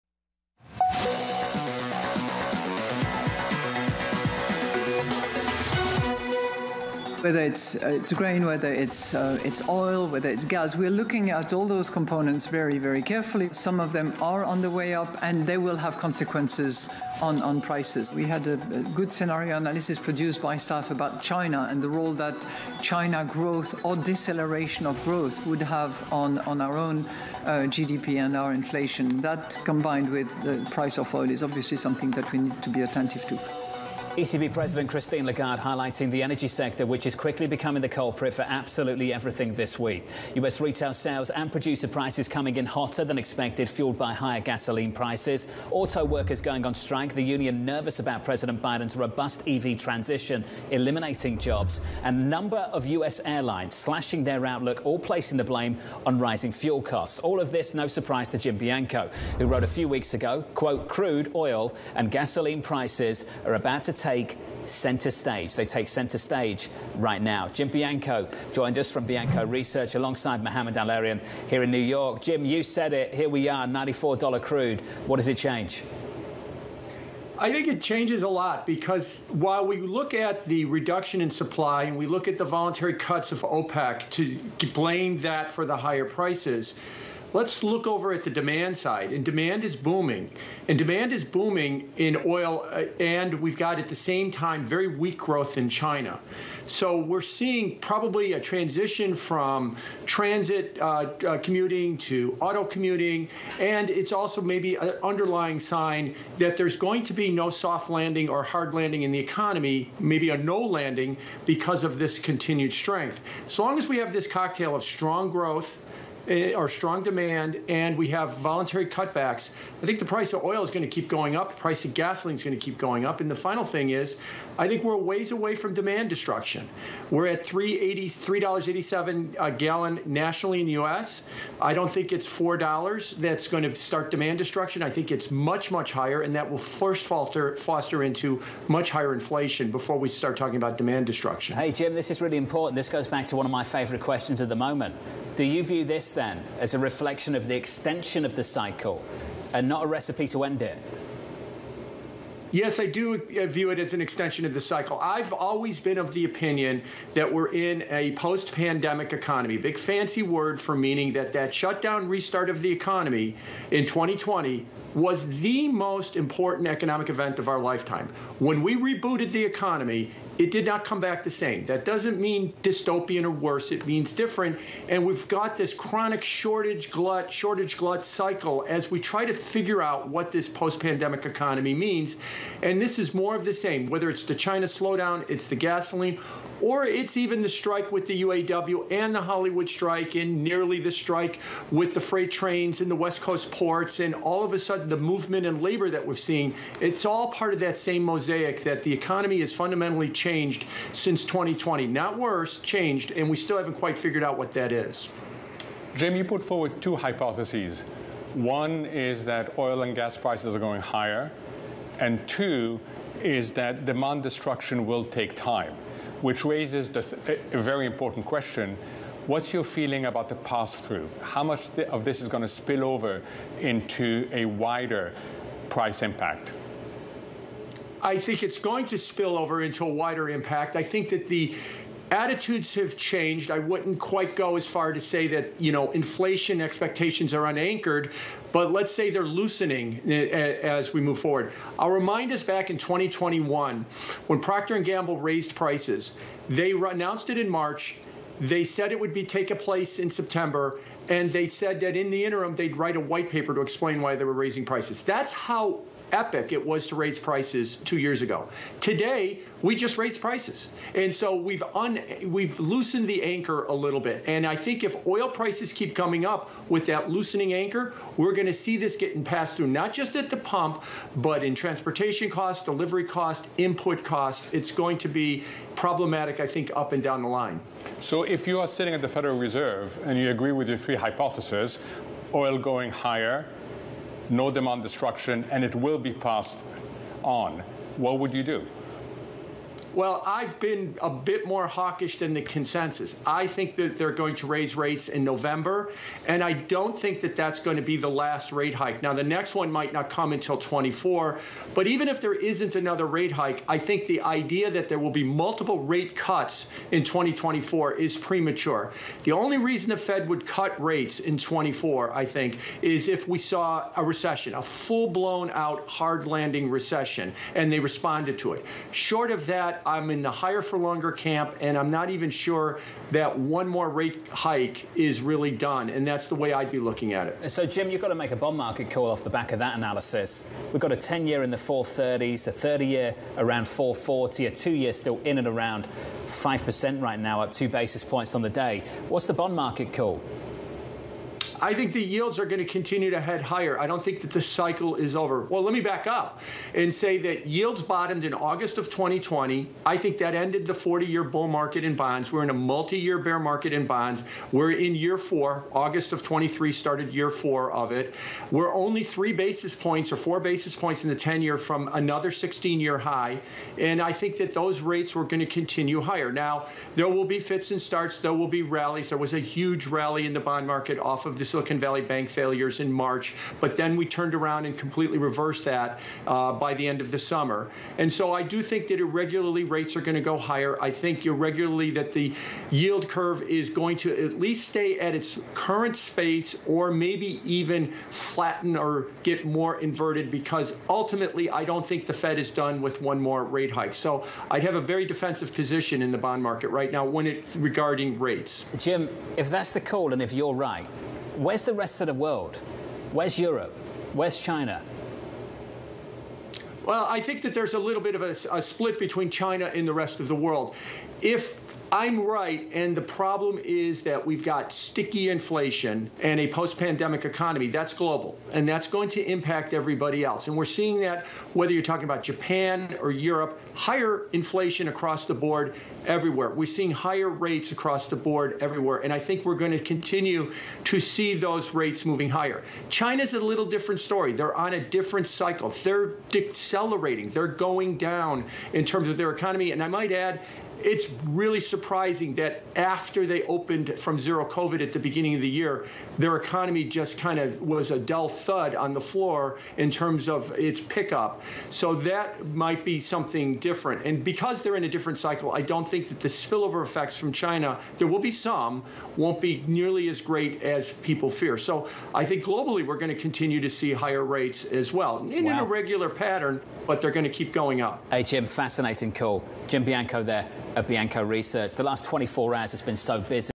Jim Bianco discusses Energy Prices, the Post-Shutdown Economy, Central Bank Policy & the Bond Market
Jim Bianco joins Bloomberg to discuss Energy Prices, Post-Shutdown Economy, Inflation Expectations, Central Bank Policy and the Bond Market with Jonathan Ferro & Mohamed A. El-Erian.